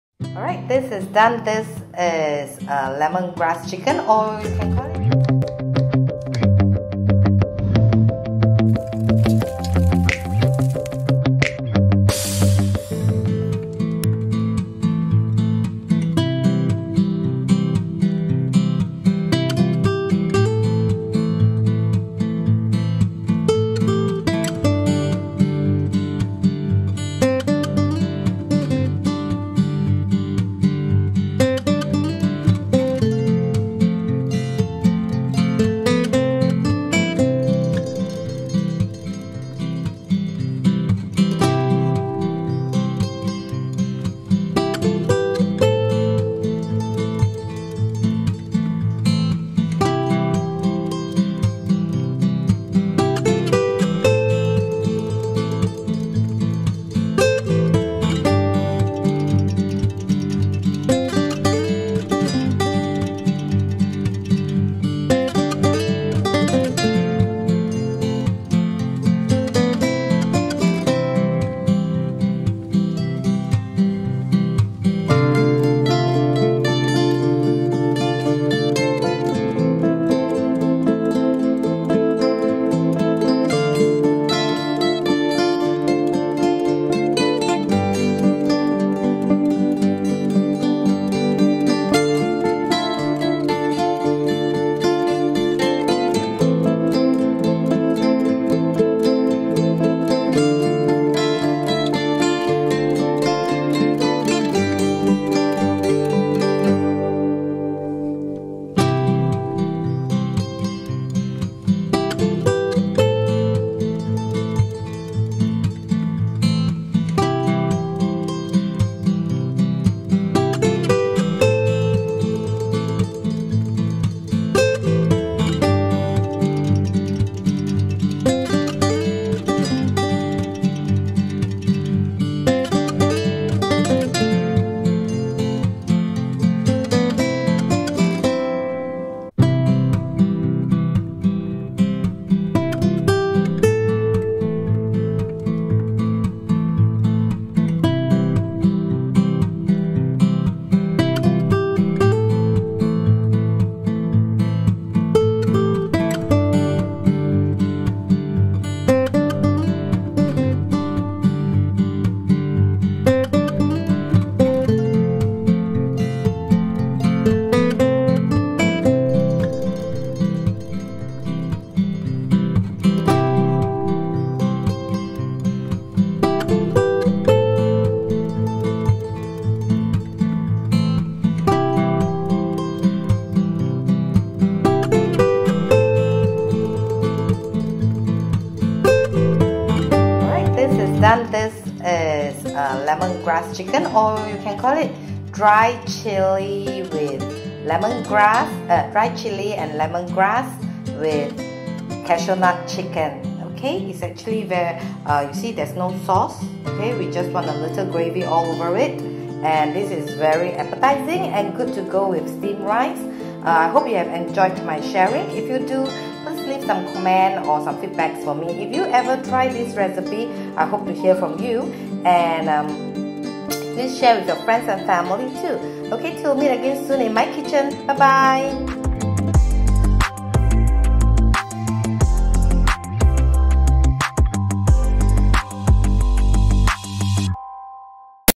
Lemongrass Chicken with Cashew Nuts | Dried Chilli Chicken with Cashew Nuts
Sharing with you today a very aromatic dish of Lemongrass Chicken with Cashew Nuts. I have added in dried chicken for extra flavour. You can omit that if you are not into spicy food. Some of you may know this dish as Dried Chilli with Cashew Nuts as you see it in the menu in restaurants.

As some of you may know me that I am a freak over herbs and spices. Lemongrass is one of herbs that I used quite a bit in my daily cooking. I love the lemony smell. It just give every dish a very aromatic and fresh smell. It will make the food taste so good.

This is a very simple to do dish, just need a little effort as we need to first marinate the chicken, then fried it only after we prepare the sauce. It may sounds lot of work but trust me...NOT AT ALL, it will take so little time and it is very simple and easy preparation too

So do check it out. If you try this recipe please do share some feedback with me. I love to hear from you

Click on the notification button too, this way you will always be on top of my weekly video recipes.

Thanks